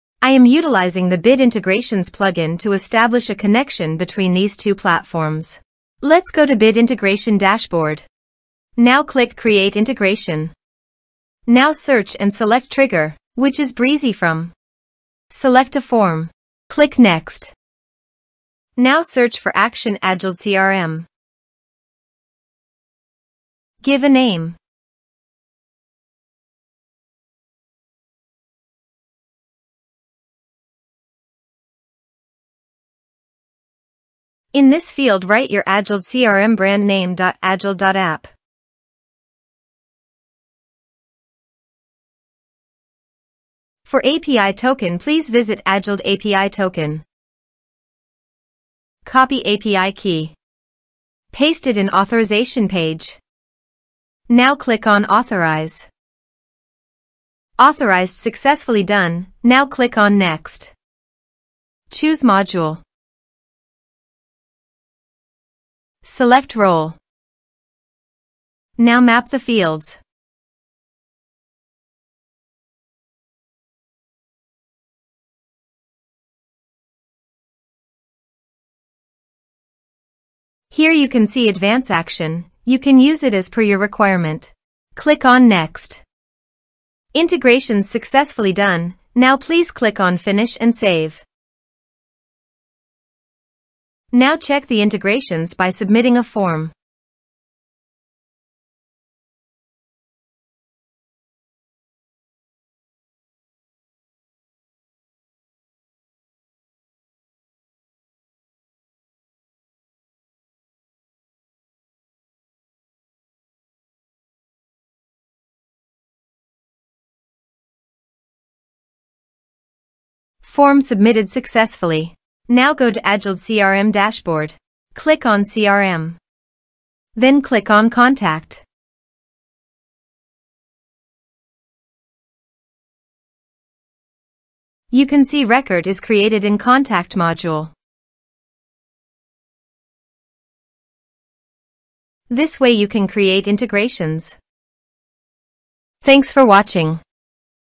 Integrating Brizy Form with Agiled CRM | Step-by-Step Tutorial | Bit Integrations
🚀 Elevate your data management game with our comprehensive tutorial on integrating Brizy Form with Agiled CRM using the powerful Bit Integrations WordPress plugin. Discover how to seamlessly sync your Brizy Form data, submissions, and form responses with Agiled CRM, optimizing your data management efforts and harnessing the potential of a versatile CRM platform!

Join us in this enlightening video as we guide you through the step-by-step process of integrating Brizy Form with Agiled CRM, harnessing the capabilities of the Bit Integrations WordPress plugin. Unleash the power of data automation and take your form submissions and data tracking to the next level by leveraging the efficiency of Agiled CRM.

🌟 Key Highlights:
✅ Detailed walkthrough of integrating Brizy Form with Agiled CRM using Bit Integrations for WordPress.
✅ Optimize your data management strategies with seamless integration and synchronization.
✅ Enhance your WordPress website by effortlessly linking your Brizy Form submissions and form responses to Agiled CRM for better data tracking.

🔗 Whether you're a blogger, business owner, or data enthusiast, this integration empowers you to effortlessly manage, track, and sync your Brizy Form data and submissions with Agiled CRM. Join us now to experience the efficiency of seamless data integration!

🎥 Ready to enhance your data management strategy? Join us in mastering the art of seamless data integration! 👍